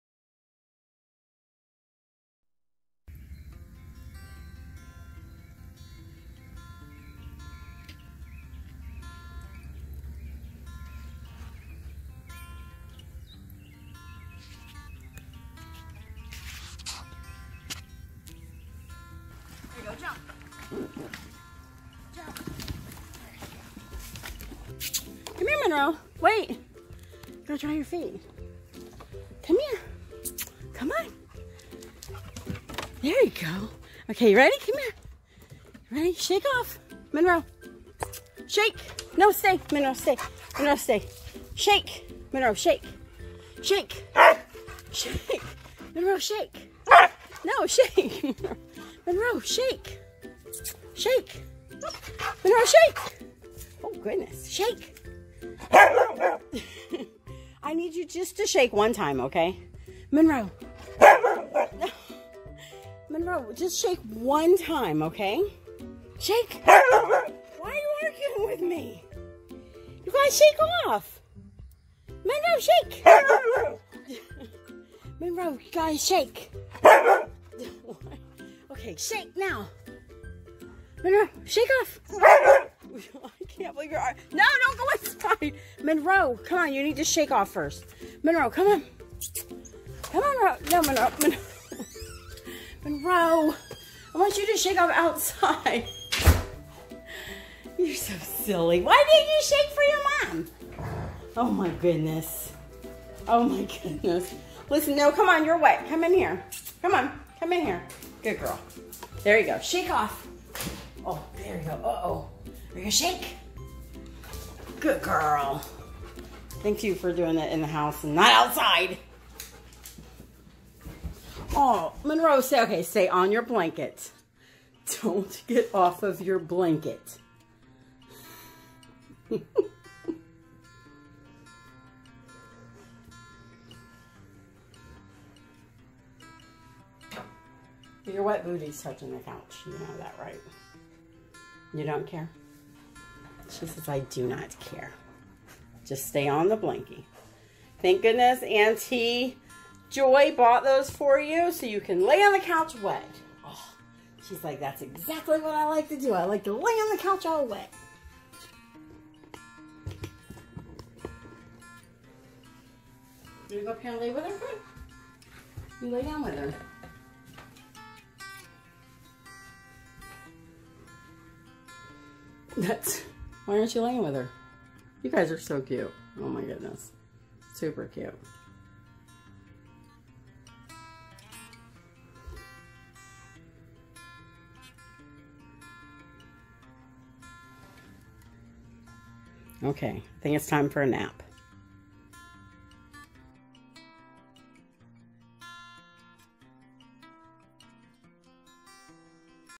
Husky Argues About Shaking Off! Opens Door & Jumps On Couch!
After Monroe was done floating she got out of her boat and refused to shake off! She said her Daddy said she wasn't allowed to shake her booty. lol Then she opened the door and went inside! She jumped on the couch to dry off ❤️😊👍! We would Love for you to join our YouTube Family!